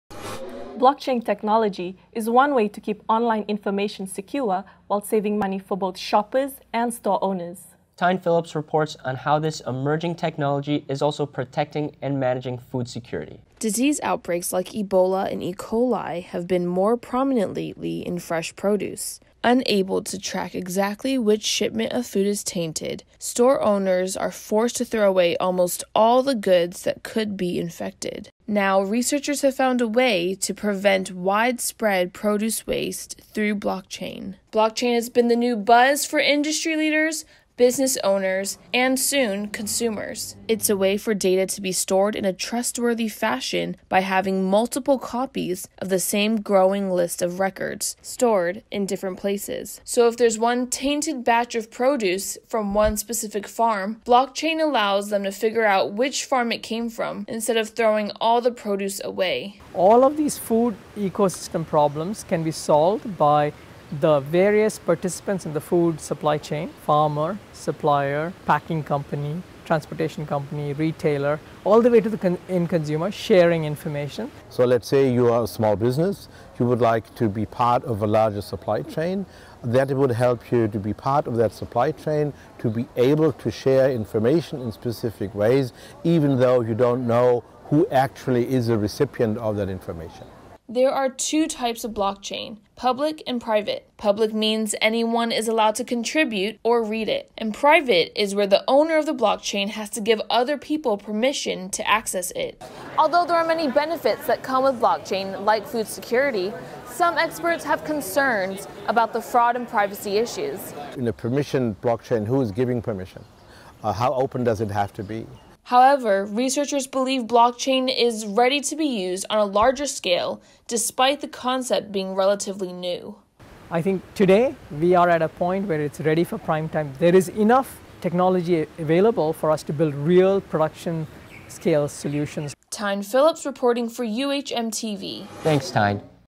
HICCS-Blockchain
Experts from the UH Manoa Shidler College of Business HICCS-52 conference discuss the emerging technology of blockchain and how it can be used to assist in food security. Tyne Phillips reports for UHMTV.